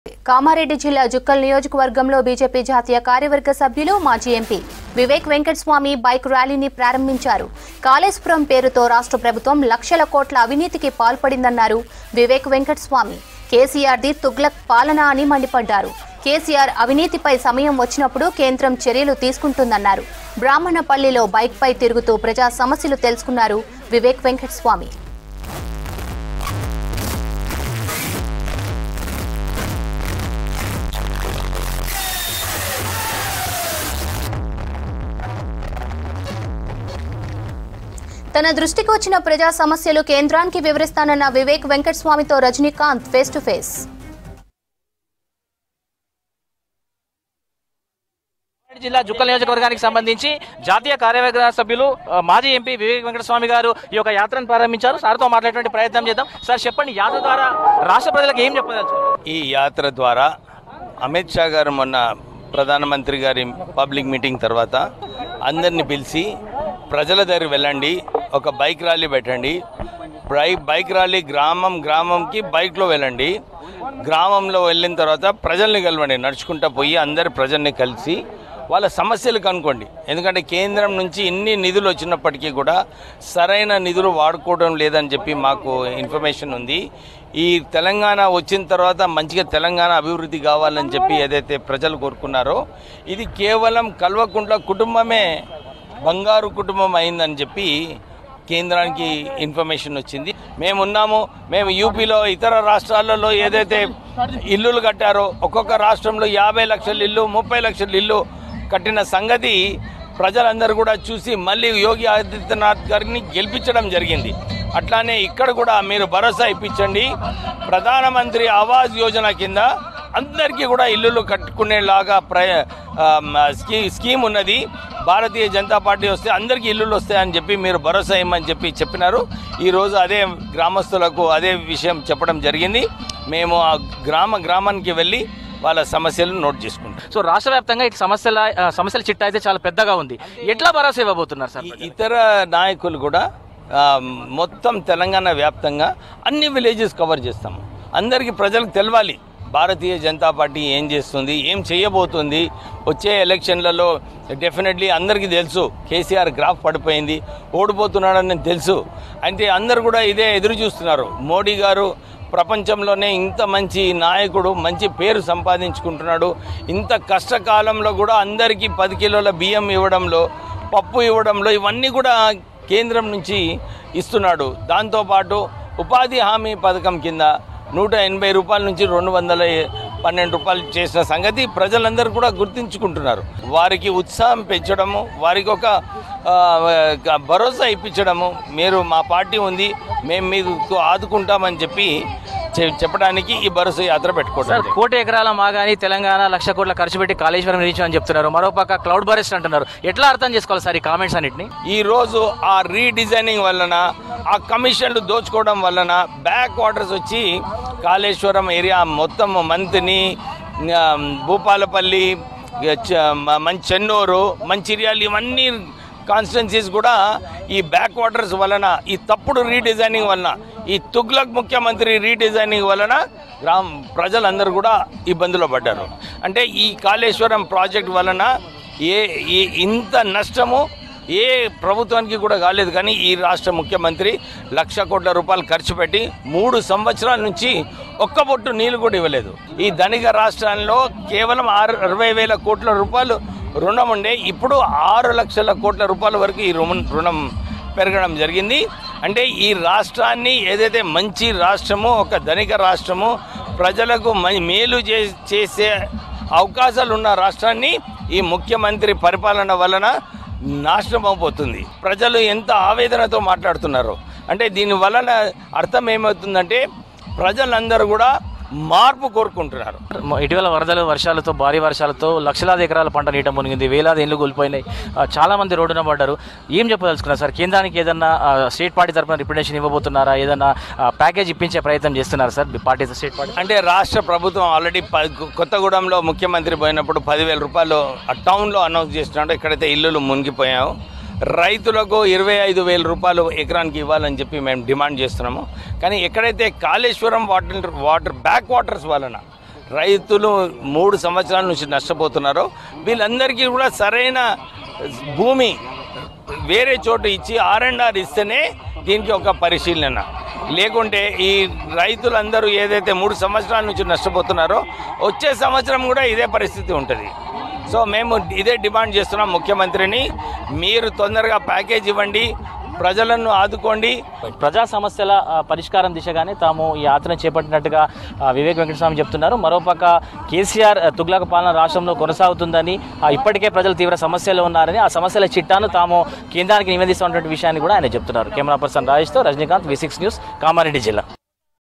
BJP Leader Vivek Venkataswamy Starts Bike Rally In Kamareddy | Praja Gosa - BJP Bharosa | V6 News
BJP National Executive Leader Vivek Venkataswamy Starts Bike Rally In Kamareddy | Praja Gosa - BJP Bharosa | V6 News